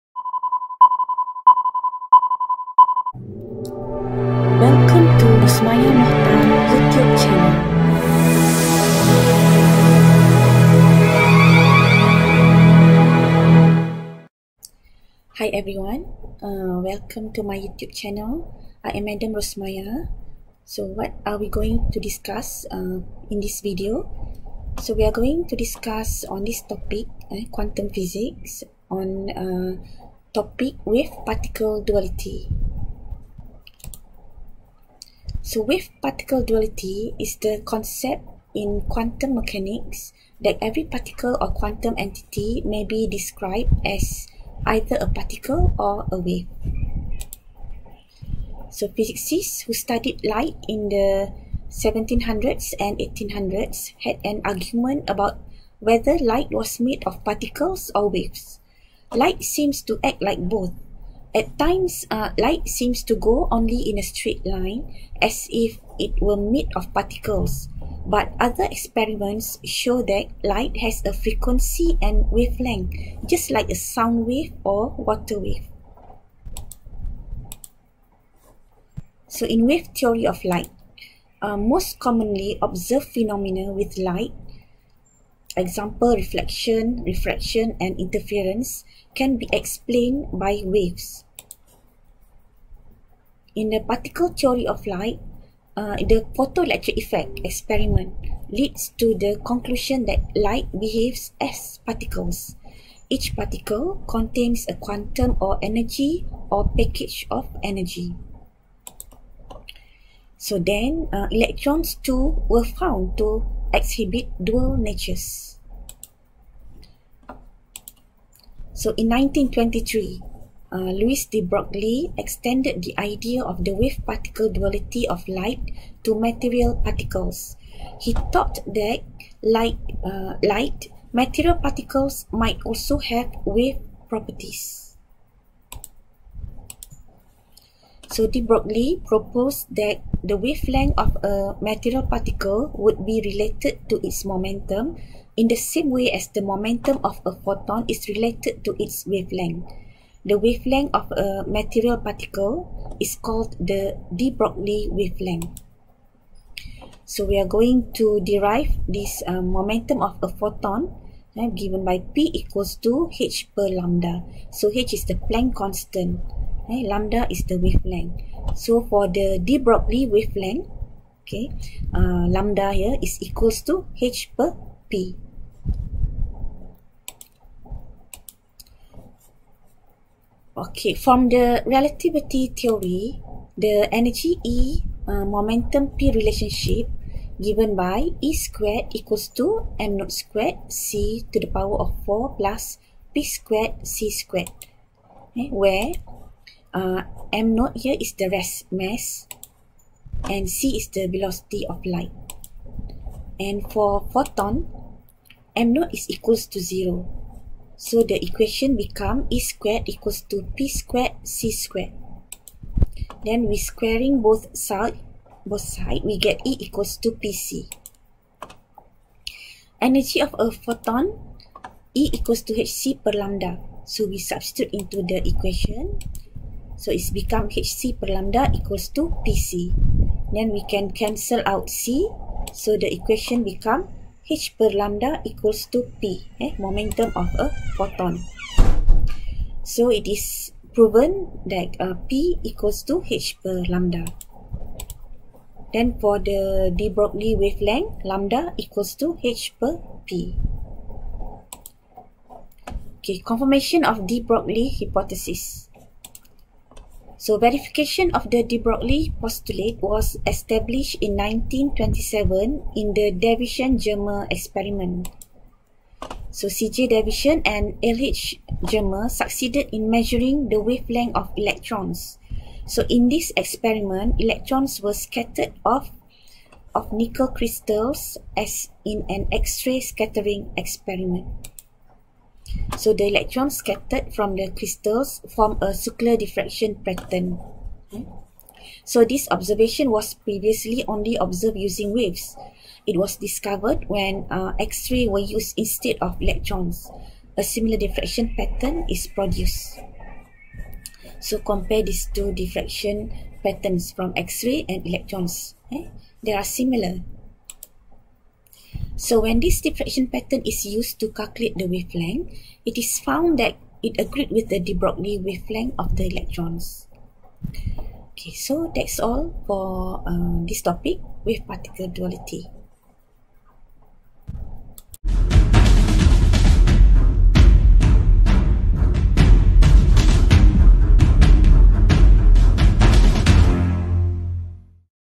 Quantum Physics - Wave Particle Duality
Wave–particle duality is the concept in quantum mechanics that every particle or quantum entity may be described as either a particle or a wave.